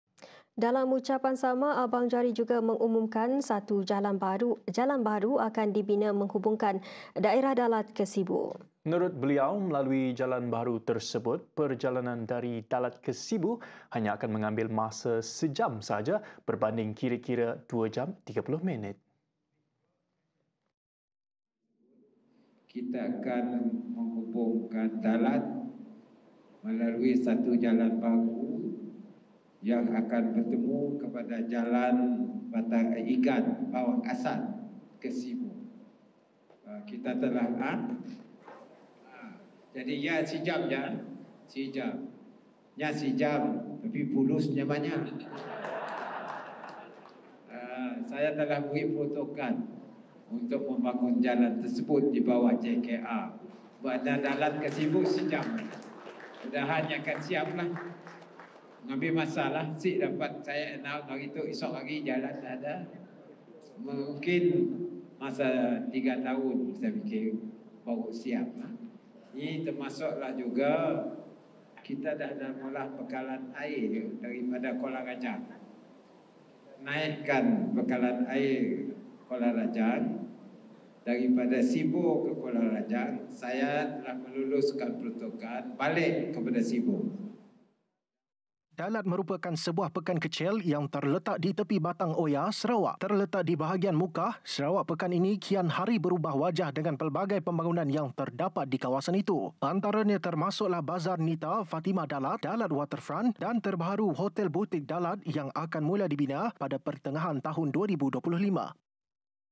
Jalan baharu hubungkan Dalat dan Sibu akan dibina - Abang Johari
Premier Sarawak, Datuk Patinggi Tan Sri Abang Johari Tun Openg mengumumkan sebuah jalan raya baharu yang akan menghubungkan Daerah Dalat ke Bahagian Sibu akan dibina.

Inisiatif itu menurut beliau bakal menyingkatkan tempoh perjalanan iaitu kira-kira 2 jam 30 minit kepada hanya satu jam sahaja.